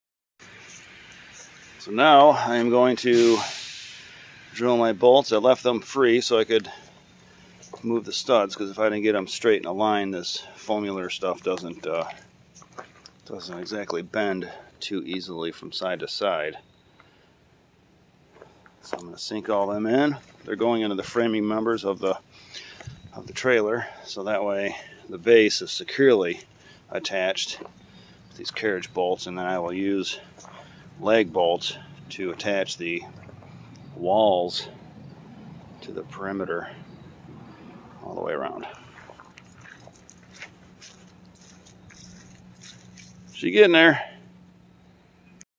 How the floor gets attached to the trailer so it doesn't blow off
Now that the foam board is laid between the floor joists, It's time to secure it to the frame. I'm using galvanized carriage bolts with lock washers and nuts under the trailer. I'll be counter sinking the heads of the bolts so when I lay the 3/4 plywood down, the don't cause any high spots.  I also ran the brake wires under the insulation and popped them out right behind the front wheels so they have less of a chance of being hit by some road debris.